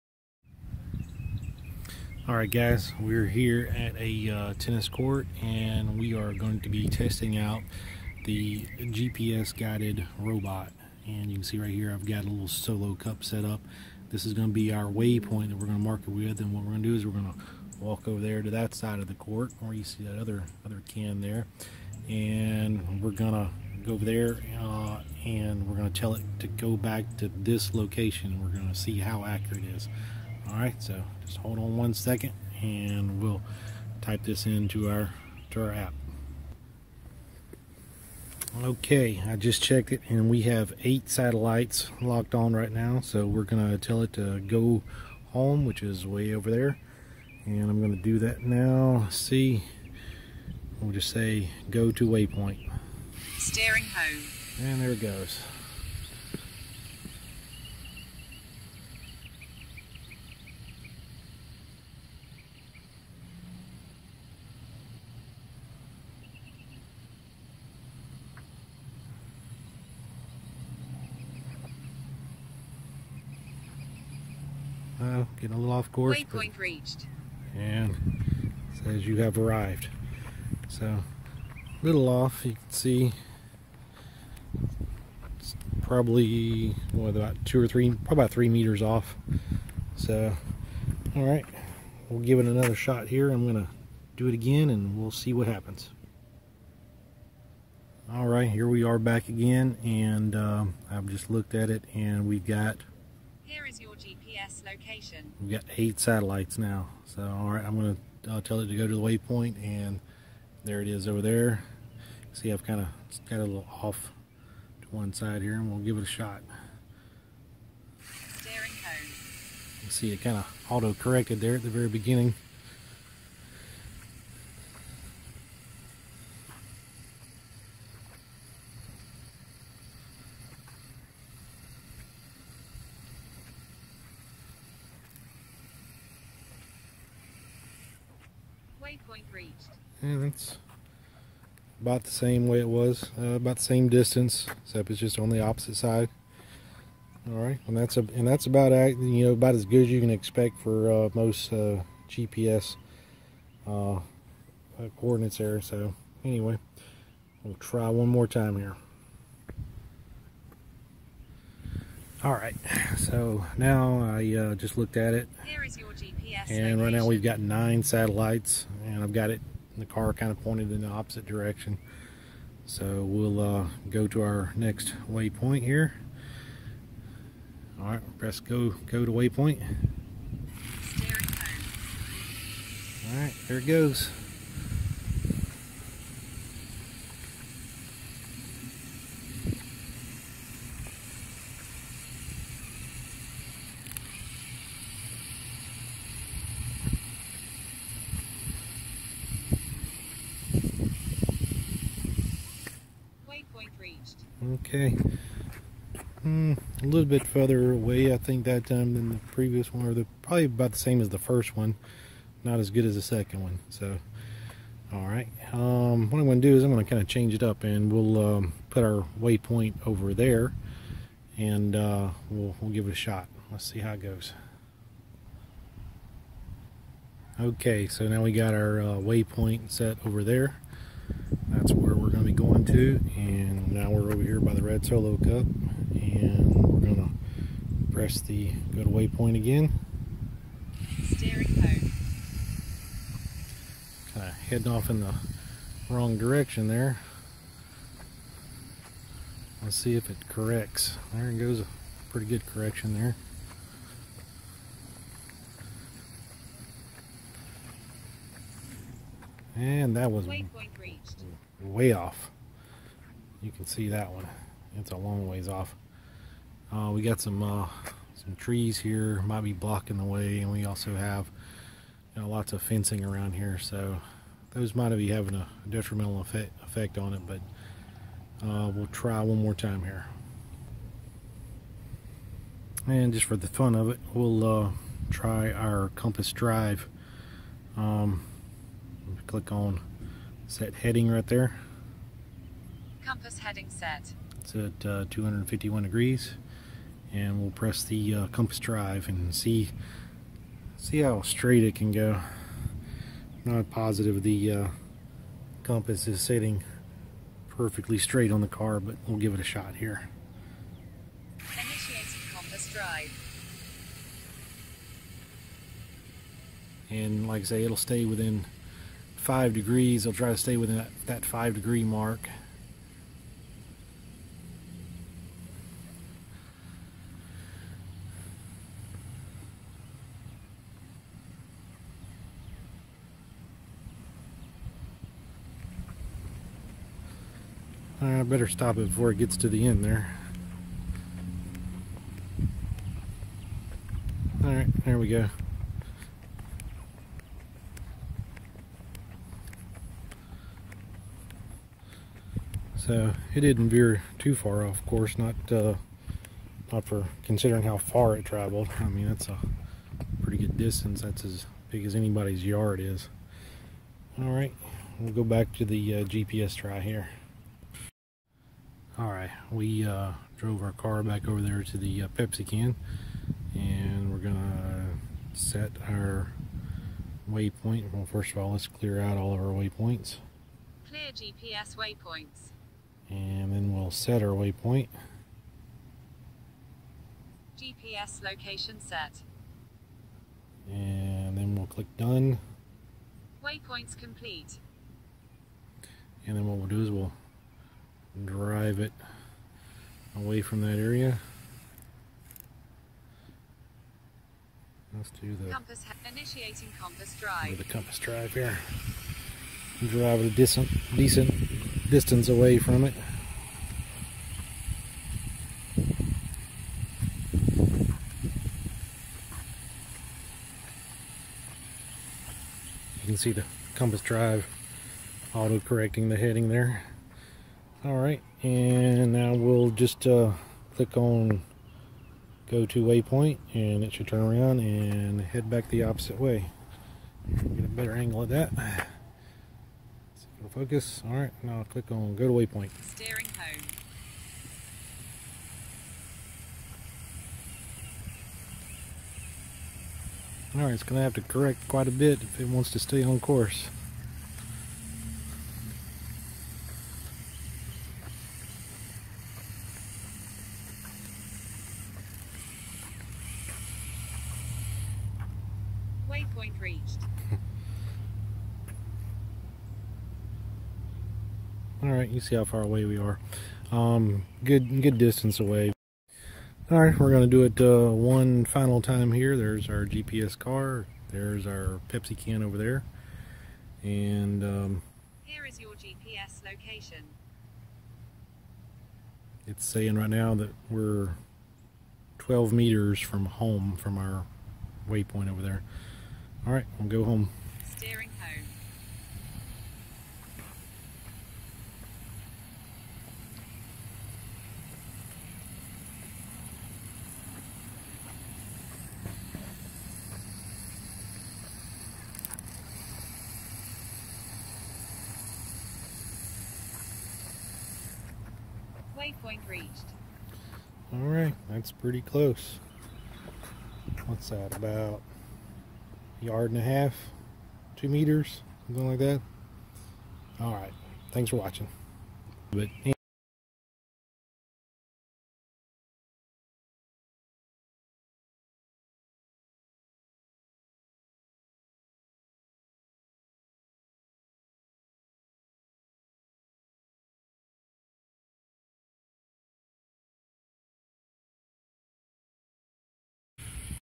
How to Build a GPS Guided Bluetooth Robot - Demo
This is a demonstration of the GPS Guided Bluetooth Robot

or you can search for it in the Gallery under "GPS Guided Robot"

You will also need to download and install several libraries referred to in the code and the Videos and Instructable

This builds upon a Bluetooth Robot that I made in a previous YouTube Video